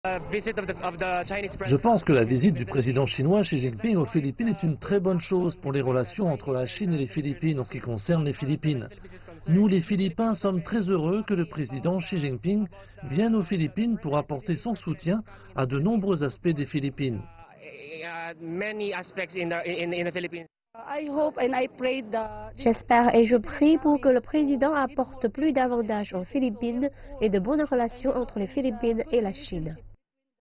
Le président chinois aux Philippines : une visite d'État saluée par la population philippine
Le président chinois Xi Jinping a été chaleureusement accueilli aux Philippines. Dans ce pays, la population estime que cette visite est une opportunité pour le développement des relations bilatérales.